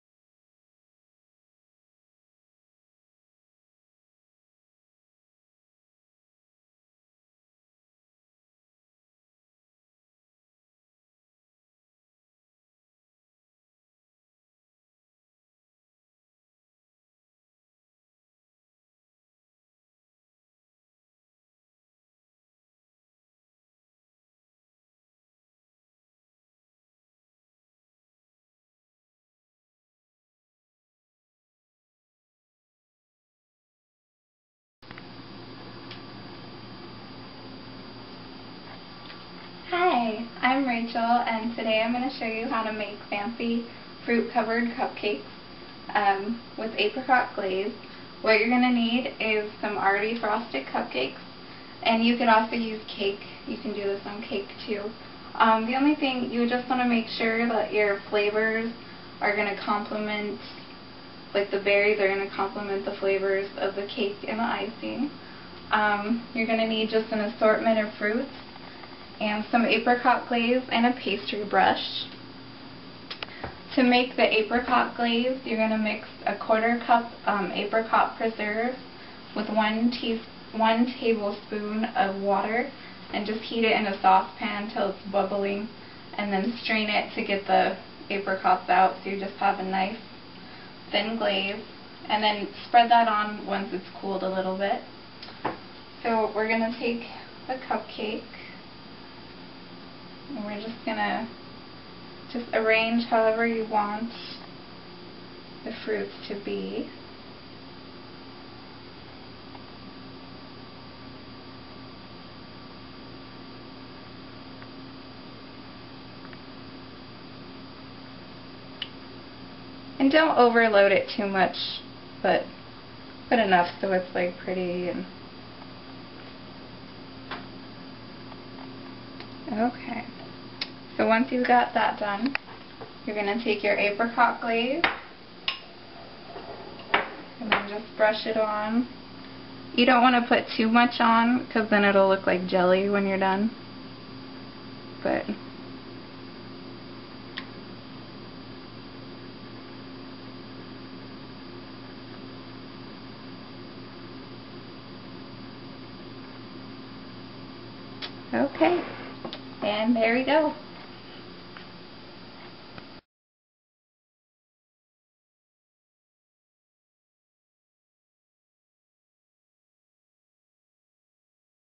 Fruit covered cupcakes
This is how you make glazed fruit for cupcakes, cakes and pastries.